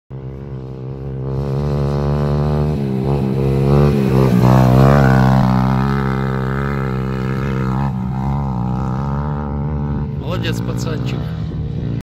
COUNTRY CROSS ФМСУ 21.09.2019 Аэродром Бородянка(40)
Кантри-кросс Федерация мотоспорта Украины. 3-й этап чемпионата Украины на квадроциклах